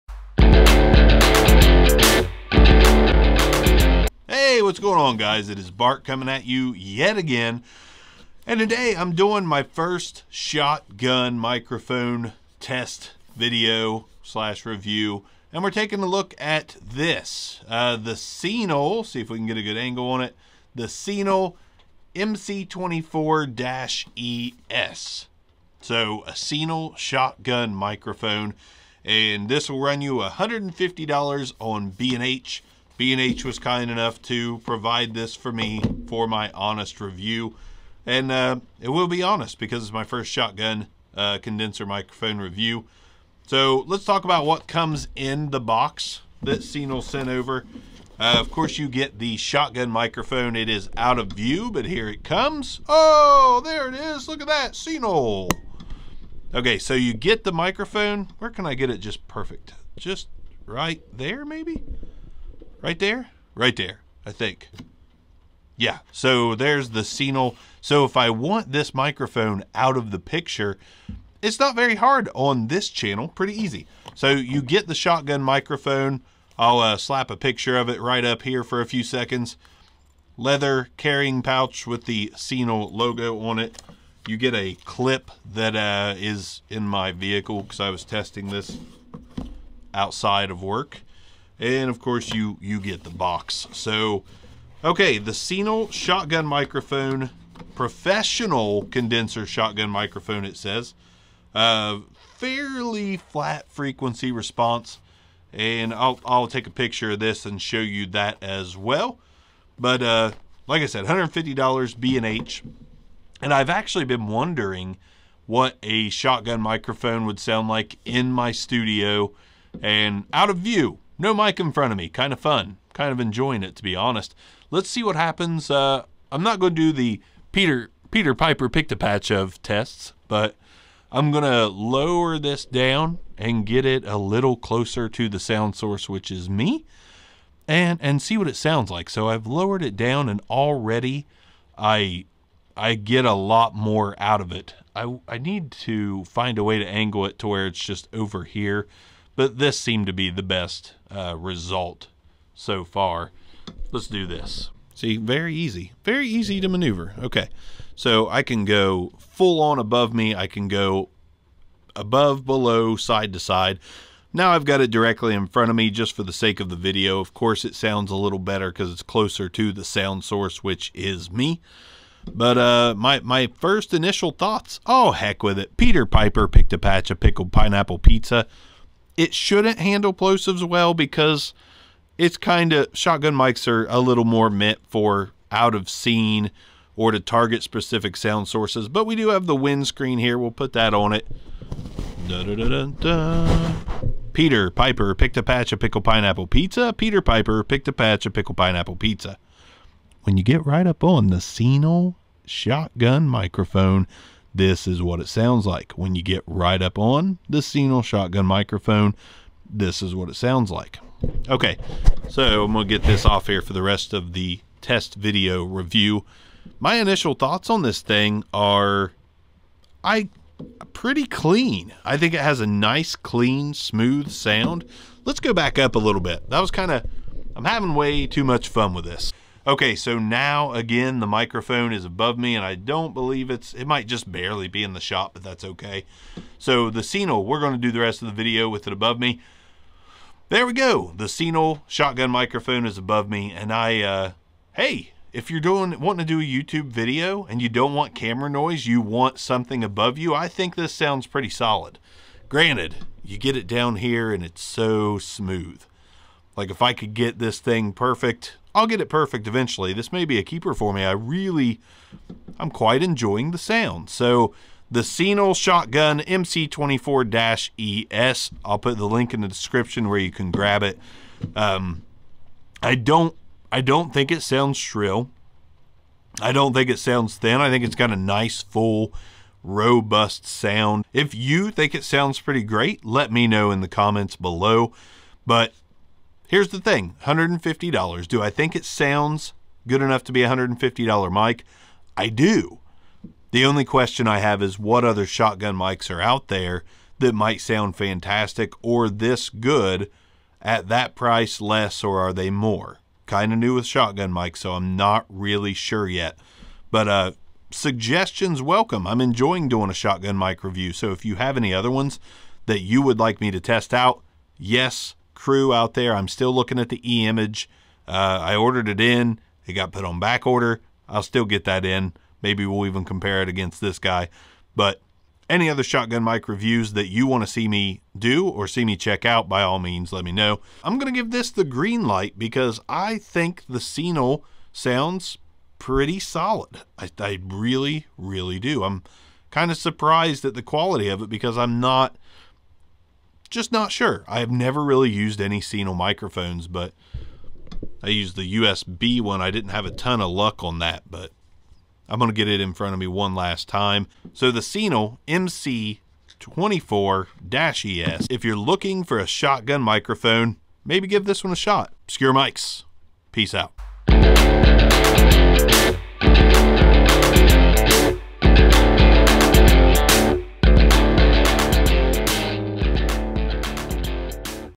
Senal MC24-ES Shotgun Condenser Microphone Test / Review
The 1st shotgun mic reviewed on this channel! And there are going to be many more in the future!

Purchase what im using!

Rodecaster Pro

Roland Black Series XLR

Vivo Boom Arm

On Stage Stationary Mic Stand